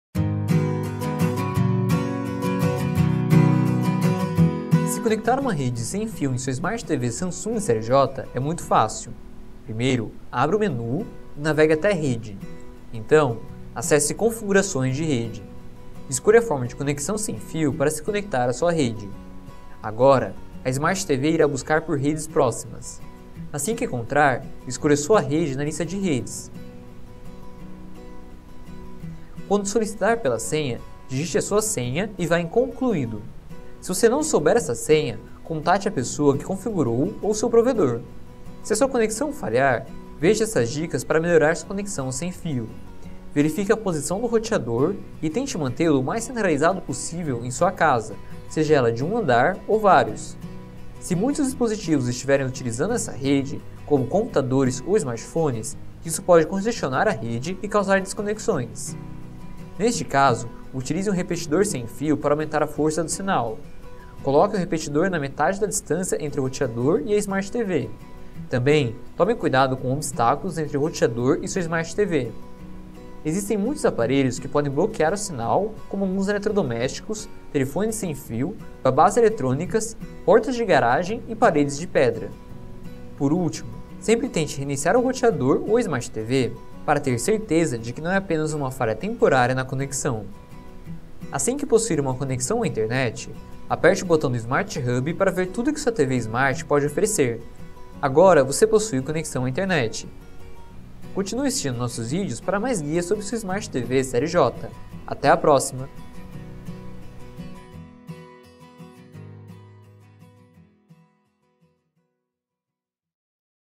Samsung | Smart TV UHD | Como conectar a uma Rede sem Fio
Com uma TV S UHD Série J, conectar-se a internet é essencial para acessar todas as funções online de uma TV Smart. Veja neste vídeo como conectar-se a internet via rede wi-fi, e como corrigir eventuais problemas ou instabilidades de conexão.